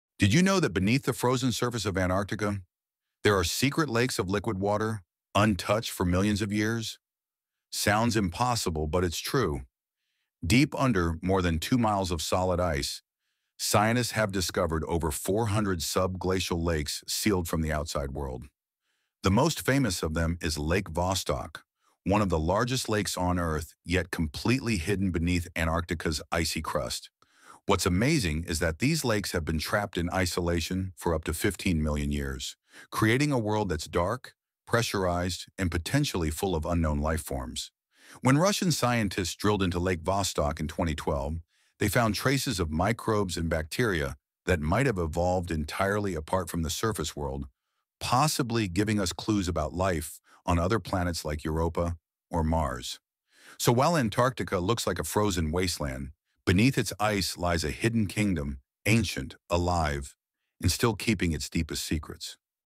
The Hidden Lakes Beneath Antarctica ❄️🌎
Did you know there are over 400 hidden lakes beneath Antarctica’s ice? One of them, Lake Vostok, has been sealed off for millions of years — and may hold life that evolved in total darkness. ❄️🧬

✅ Verified Sources:

National Geographic (2023)

Scientific American

British Antarctic Survey

NASA Earth Observatory